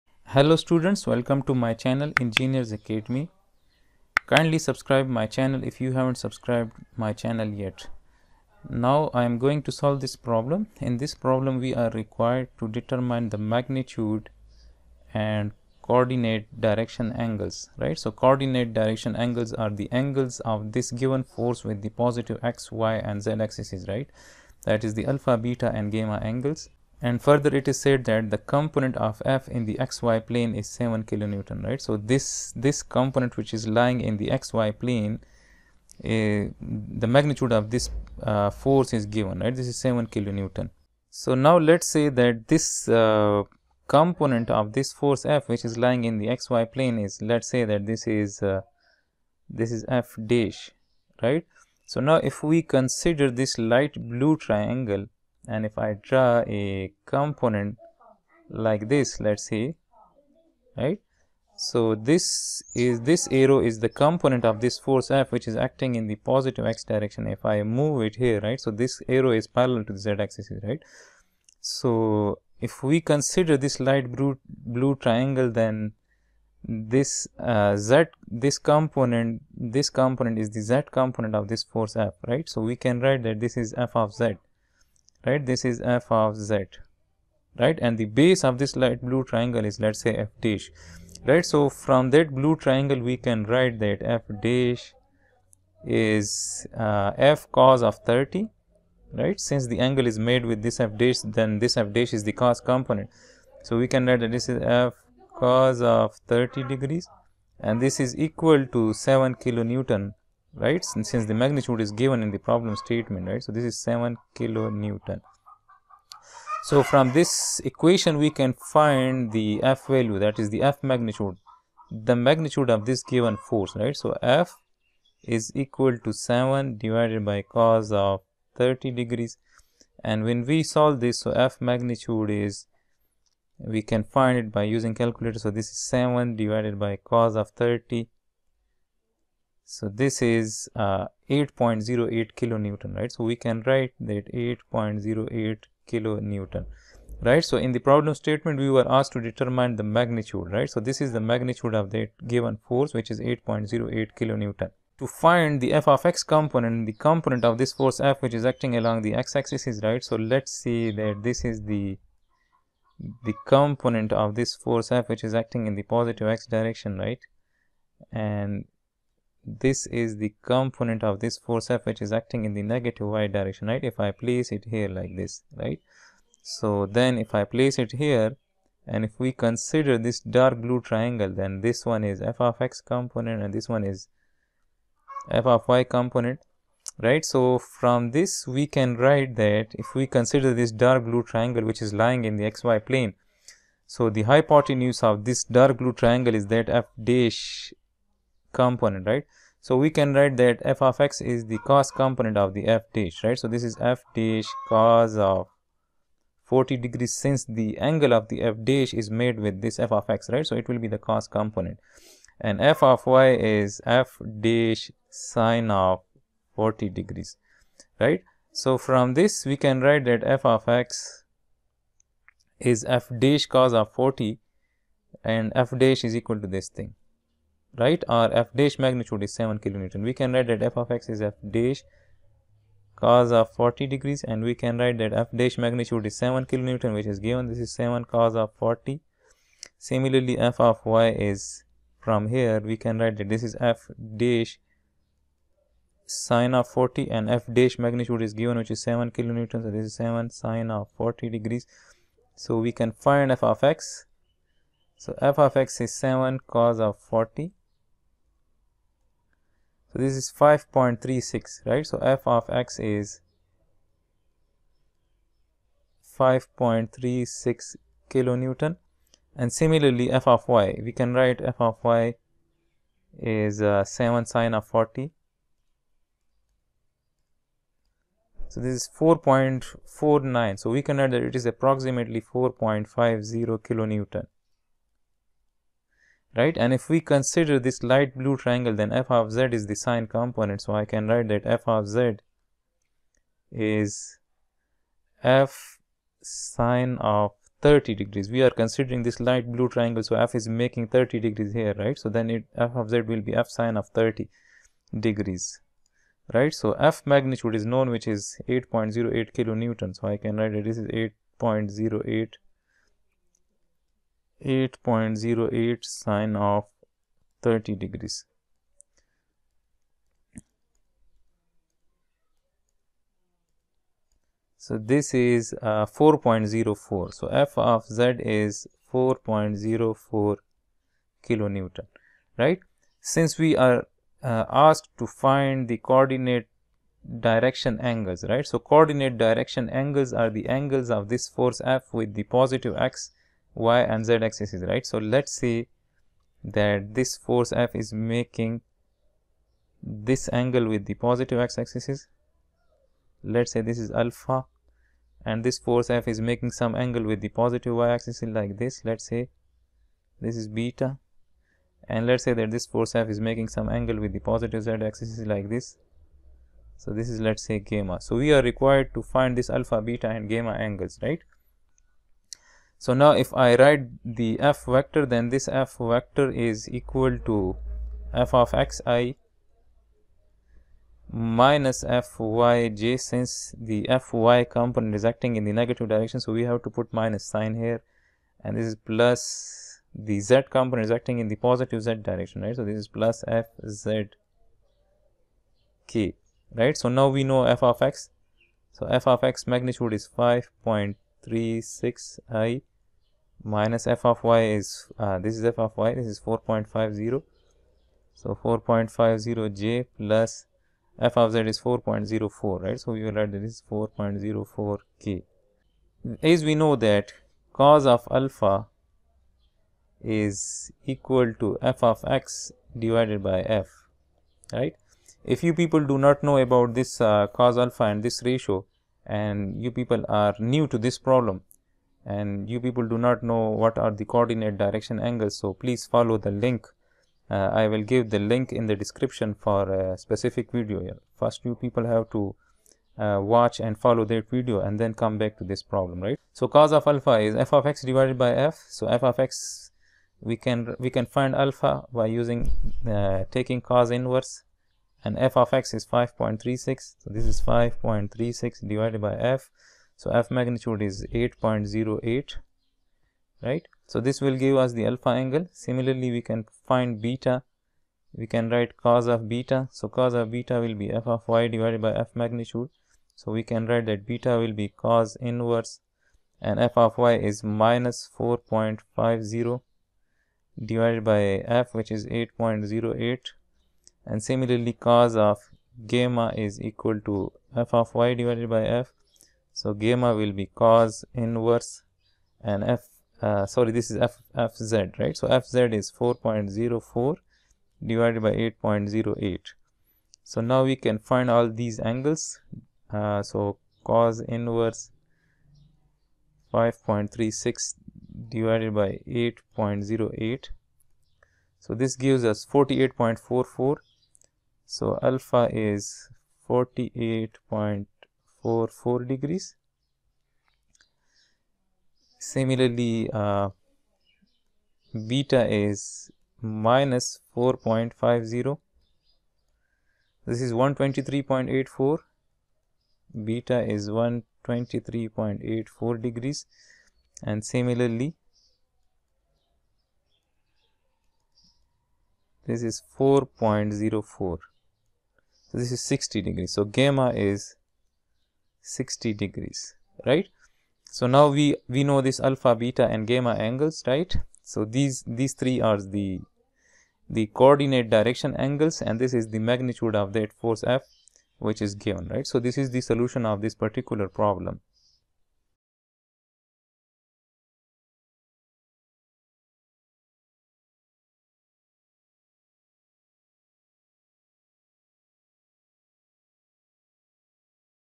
Determine the magnitude and coordinate direction angles. | Hibbeler Statics | Engineers Academy
Chapter 2:  Force Vectors

2–62. Determine the magnitude and coordinate direction angles of the force F acting on the support. The component of F in the x–y plane is 7 kN.

Cartesian Vector Representation
Magnitude of Cartesian Vector
Coordinate Direction Angles
transverse and Azimuth angles